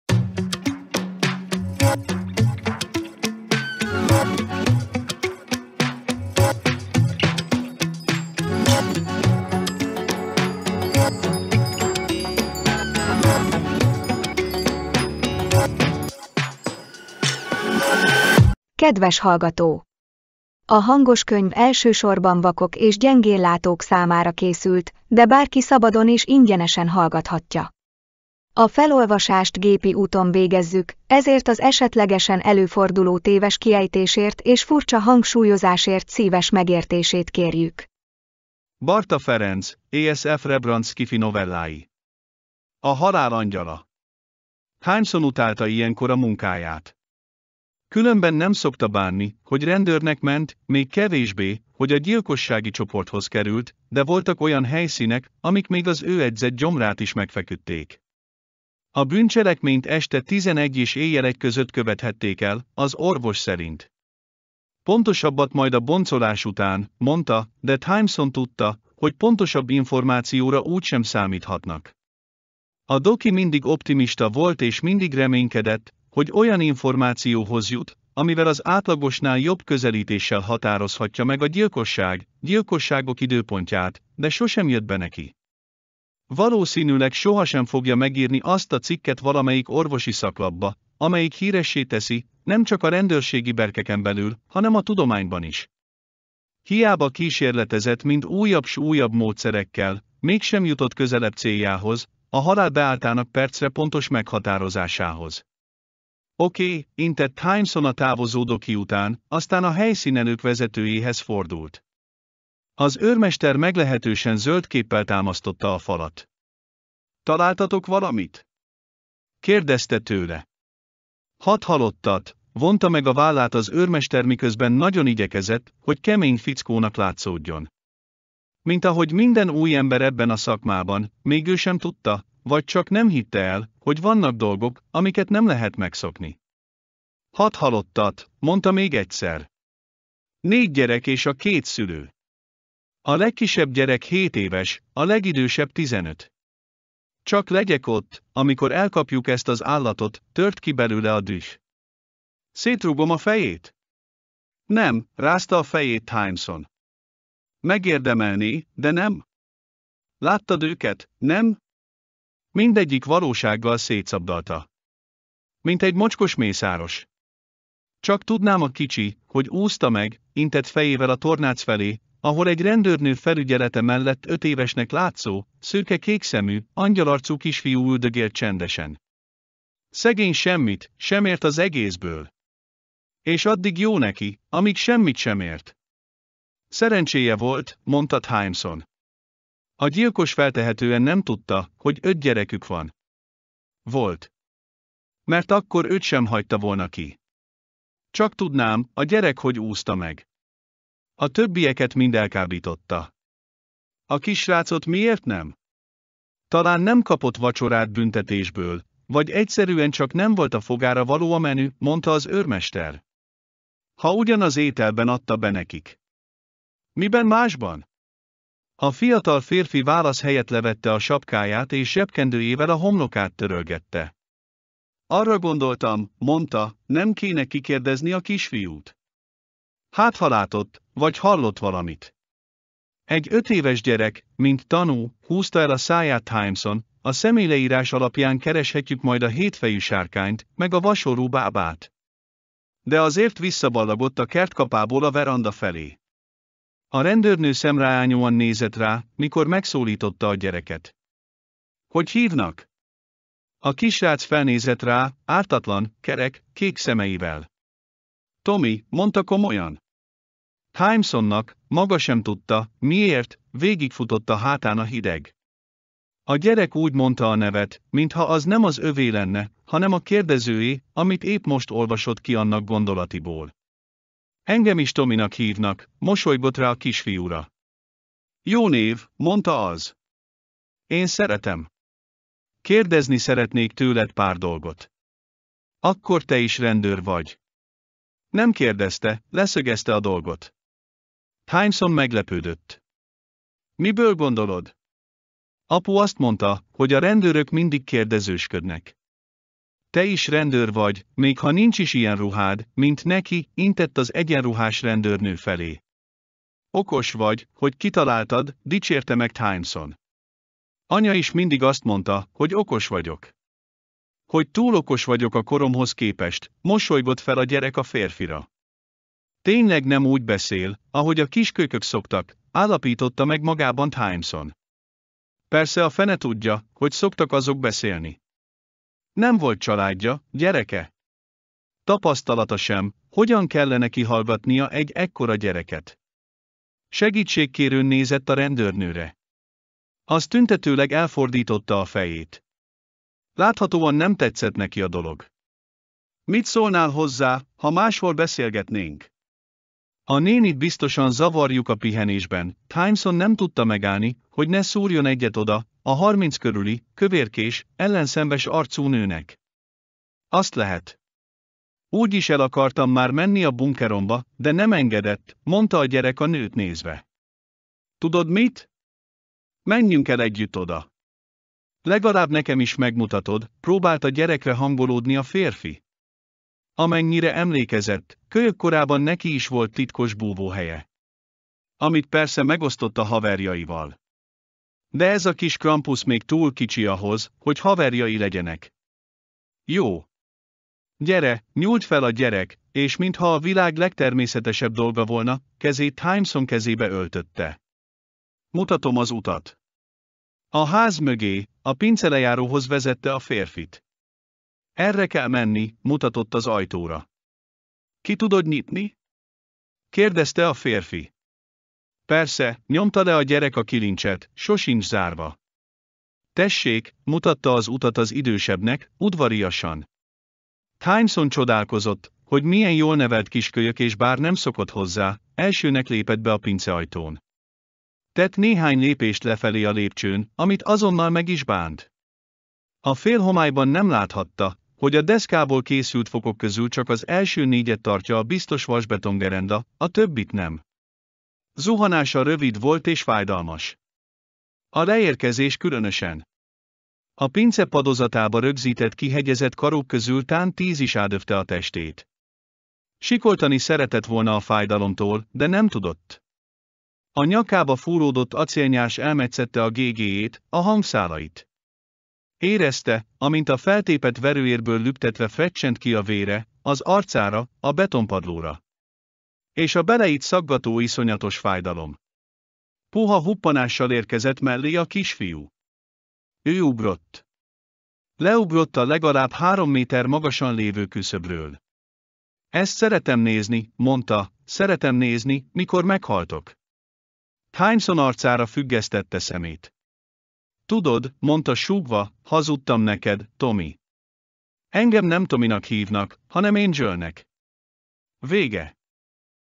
Barta Ferenc: Ace F. Rebrant sci-fi novellái - A halál angyala (Hangoskönyv)
Mától minden este felkerül egy novella a gyűjteményből. Figyeld a csatornát!

Tartalom:
A halál angyala
Kegyetlen búcsú
Gólyák
Cápaváros
A halál csatornája
A halál útja
Hétfői történet
Invaders
A lazacidény
A Lir-kristályok
Gyilkosság a mágnesvasúton
A Mars-arc
A halottasház őre
Naplopók
Otthon, papucsban
A samponok hatása az evolúcióra és viszont
Paint it black
Pitypang világa
Revans
Soha nem ér véget
System error
A szénégető
Szuperszámítógép
Az UFO-kérdés megoldása
Az utolsó metró
Ünnep
Zombi